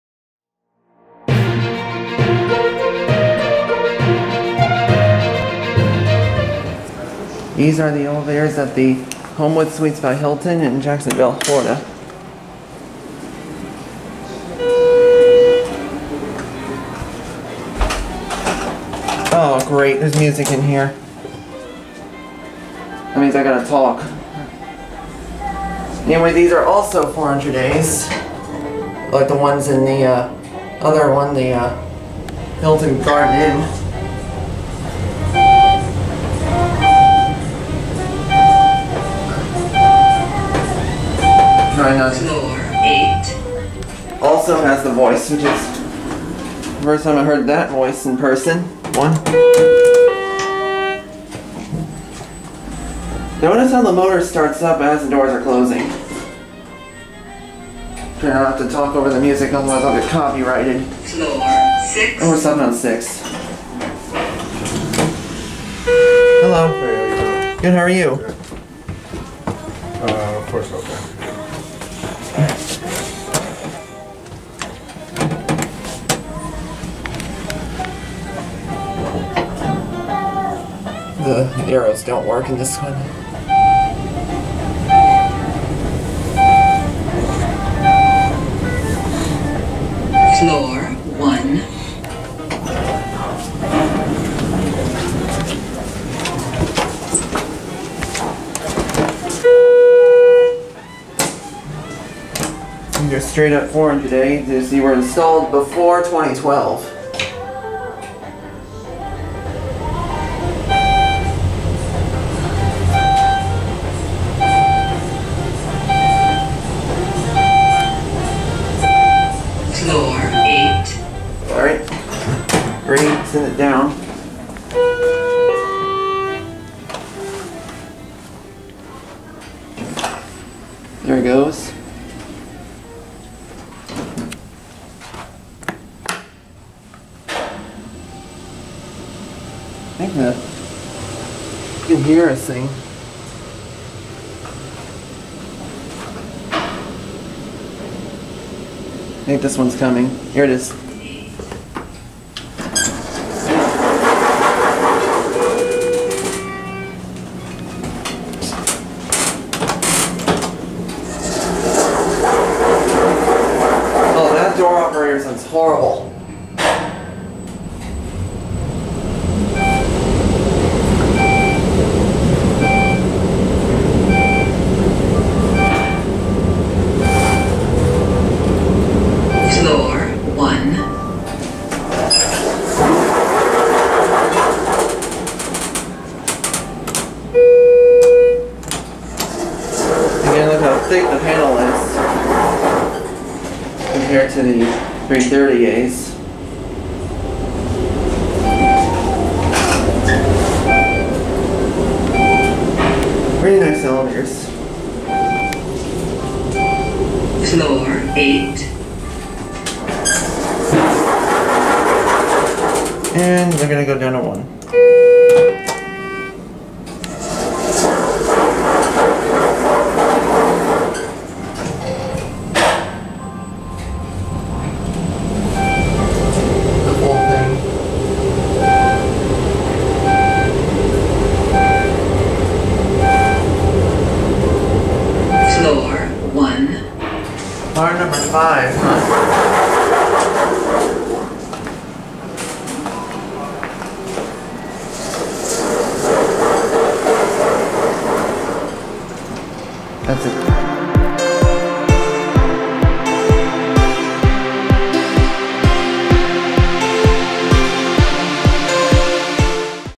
Schindler 400a Traction "CopyrightClaim-O-Vators" @ Homewood Suites by Hilton, Jacksonville FL
(9/19/20) Here are some more Schindler 400a Elevators in the same building as the other hotel. Believe it or not, these hotels are attached to each other. These elevators had music in them, earning the name "CopyrightClaim-O-Vators", like @Jowevator 3219 calls them.

Elevator Info
Brand: Schindler
Model: 400a
Call Stations: HT Premium
COP: HT Premium
Capacity: 2500 Ibs
Drive: Traction
Floors Served: 1-8
# of Elevators in Bank: 2

If Modernized
Modernizer: N/A
New Call Station: N/A
New COP: N/A

Year of Installation: 2008
Year of Modernization: N/A
Overall Rating (Scale of 1-10): 8/10

Join my Remind! Text @swflelev to 81010